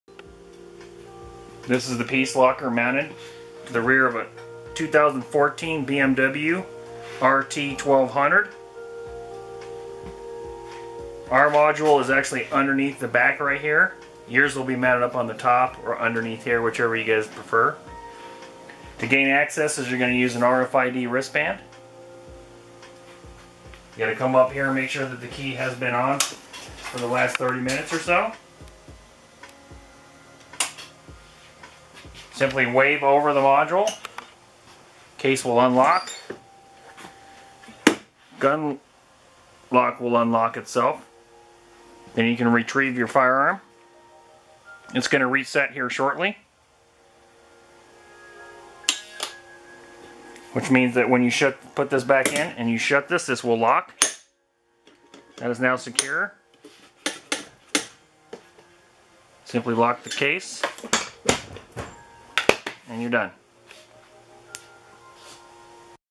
2014 BMW Piece Locker operation
This is a demonstration of the Radio Frequency Identification wristband opening the "Piece Locker" on a 2014 BMW R1200 RT.